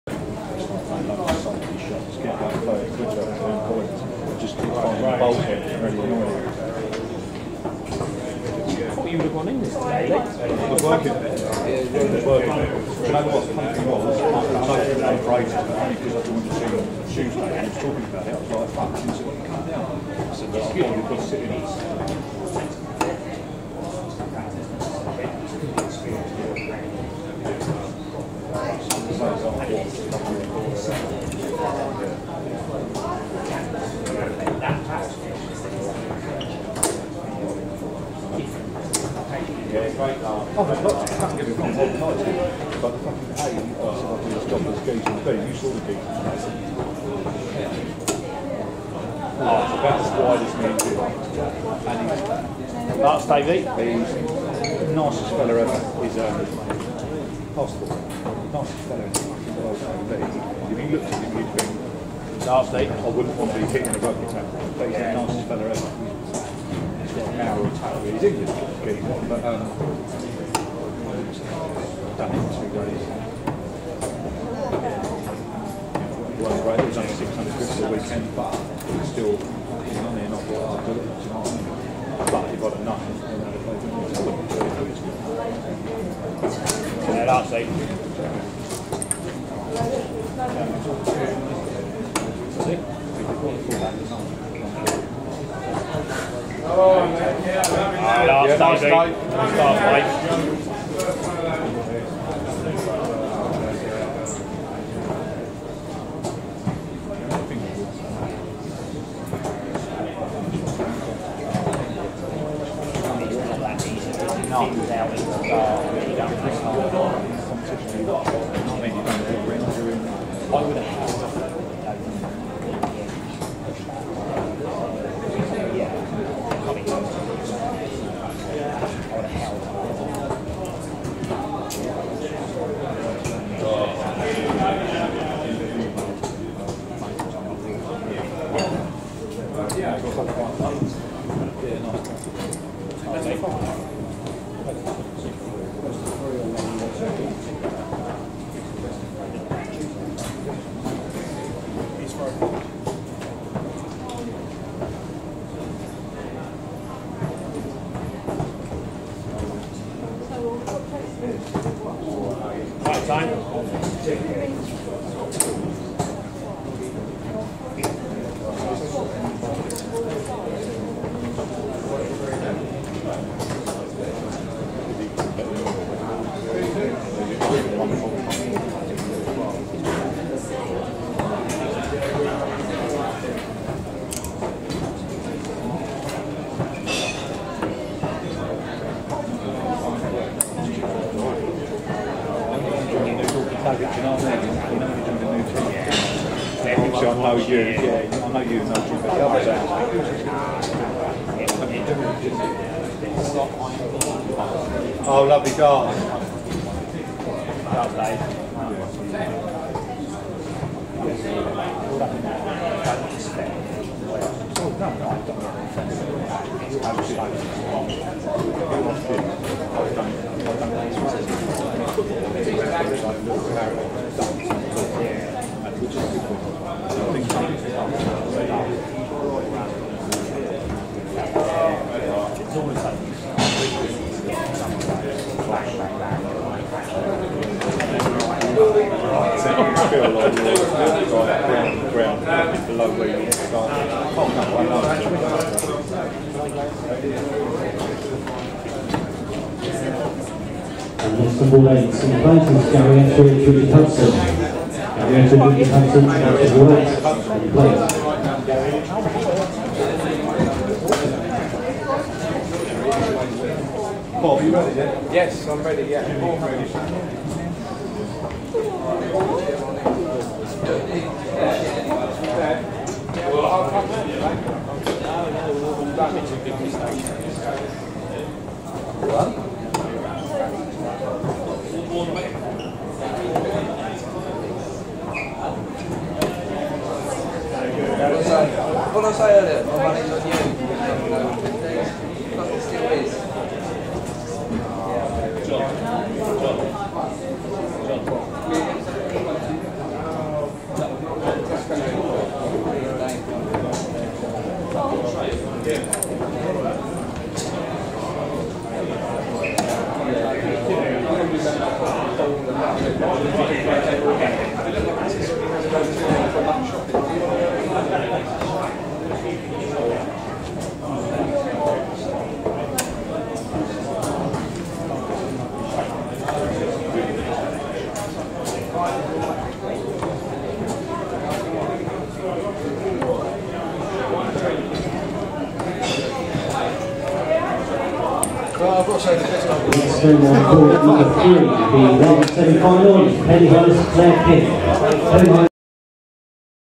Twitten Club (Brighton) Darts Open O50's Quarter-Final: Dave Hetherington vs Lee Jennings, July 2022
Twitten Club (Brighton) Darts Open O50's Quarter-Final: Dave Hetherington vs Lee Jennings, July 2022. Held at the Twitten Club (Brighton). Southwick Sports & Social Club.

Best of 3 legs.

Many thanks to Laura and Stefan for all their organisational efforts and for allowing me to film!